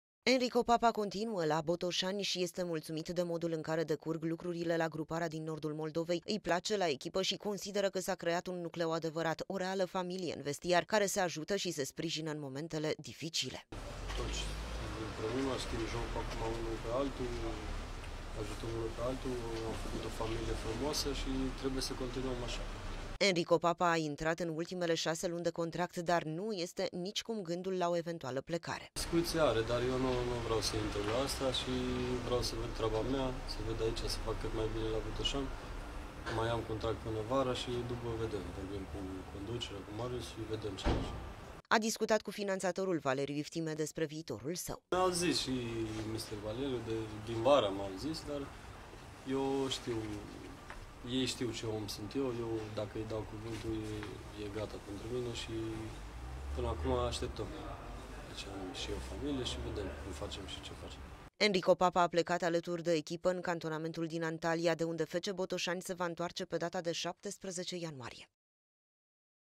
NU VREA SA PLECE DE LA BOTOSANI
Nu isi doreste sa plece de la FC Botosani. Capitanul Enriko Papa vrea sa continue sa faca meciuri bune in tricoul moldovenilor si sa se bucure alaturi de acestia de o noua calificare in playoff. A vorbit cu Valeriu Iftime despre o prelungire de contract, iar acum a plecat in Antalya in cantonament cu echipa.